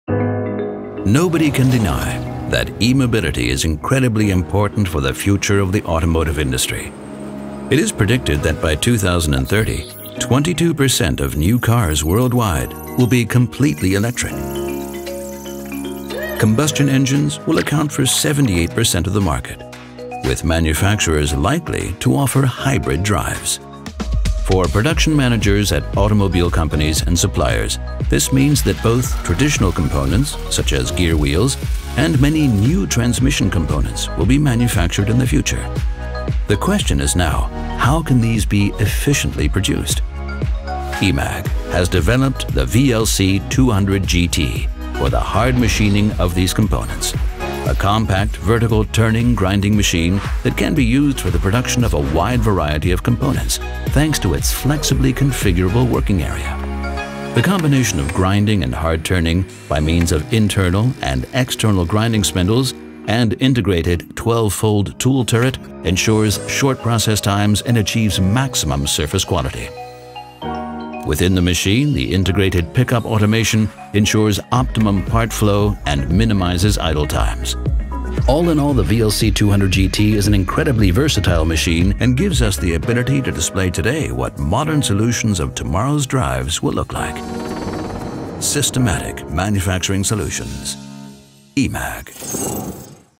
VLC 200 GT Turning Grinding Center - EMAG
In the time of e-mobility, how do we continue to meet the diversity requirements for drive technology products and the increasing production needs? The VLC 200 GT compact vertical turn-grind machine can be used for the production of a wide variety of workpieces thanks to its flexibility and configurability in the work area.